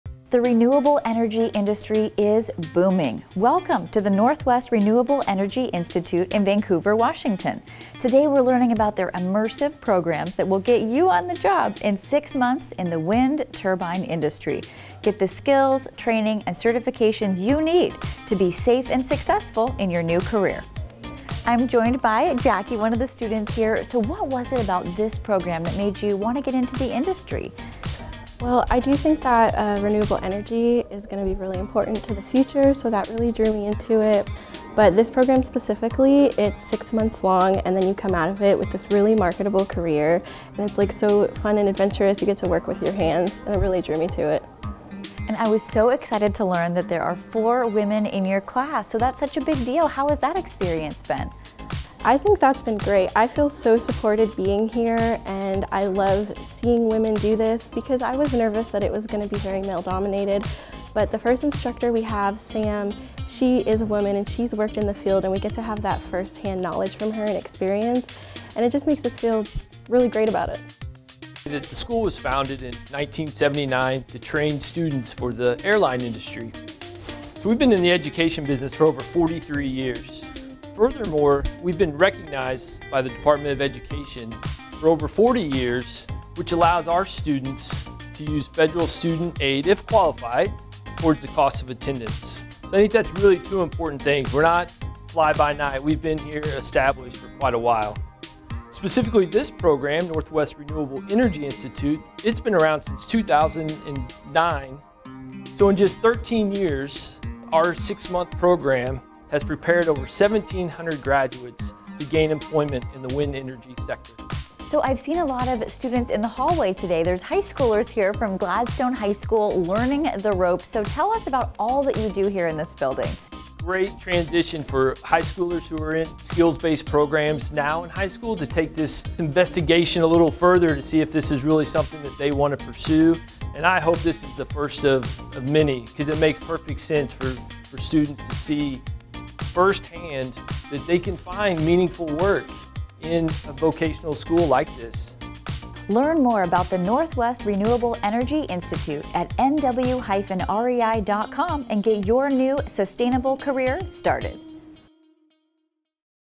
Train to Become a Wind Turbine Technician
KOIN's Everyday Northwest came out to Northwest Renewable Energy Institute to learn more about our Wind Turbine Technician Training Program.

We offer a 6-month program to prepare you to become a wind turbine technician. Our program is accredited by the ACCSC, and it's our goal to provide the technical training you need to start your career in the renewable energy industry.